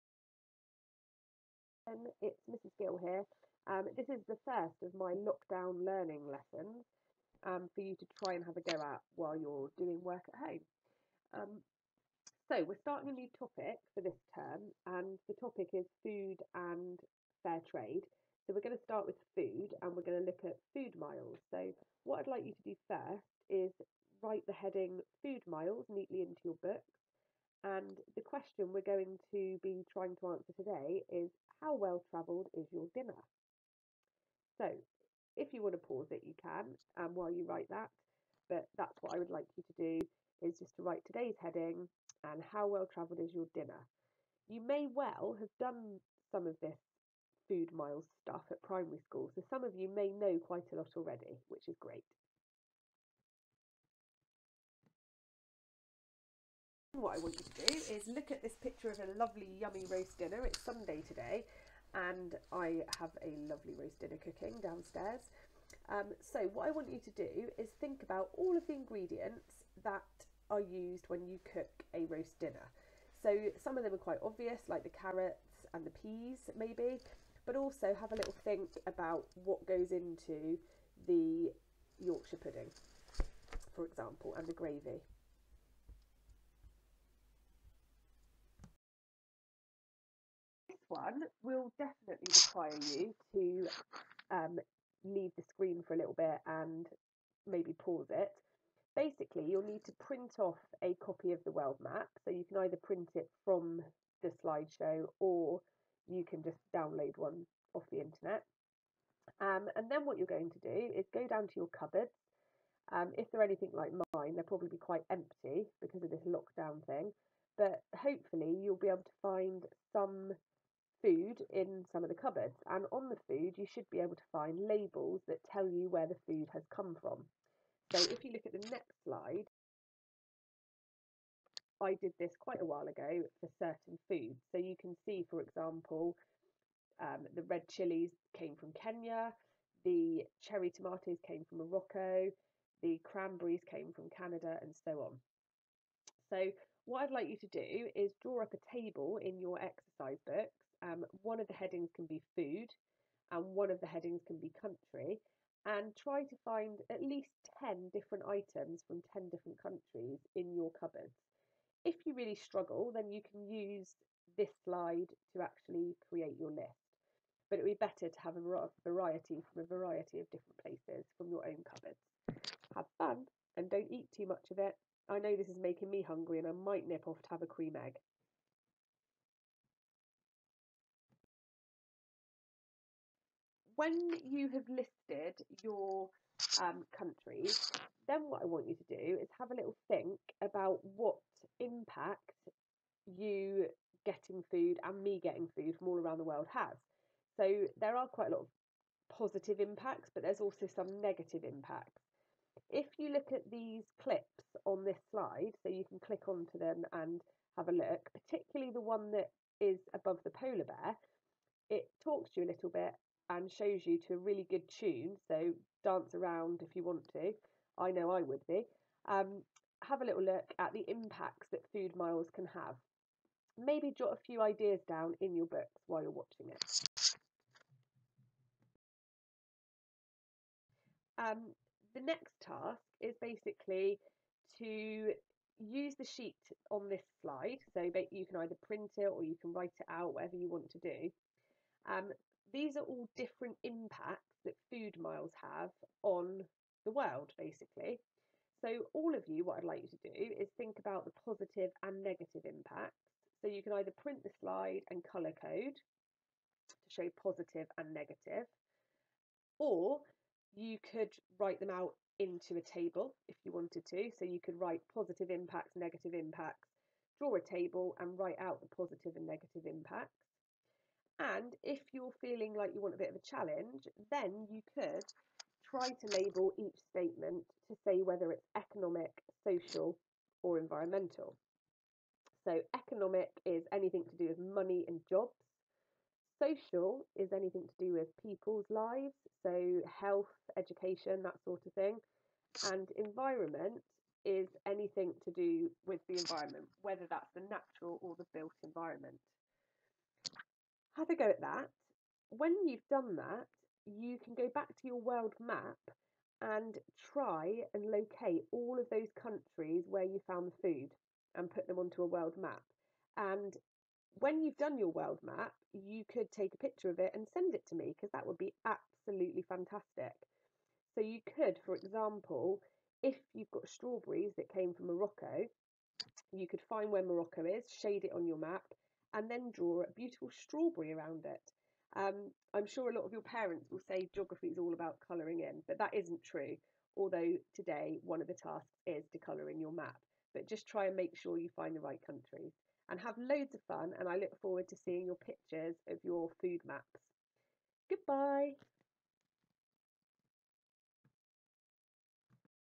01 Food Miles 1
For Year 7 students on lock down.  The learning objective is to understand what 'food miles' are and what impacts food miles have on the world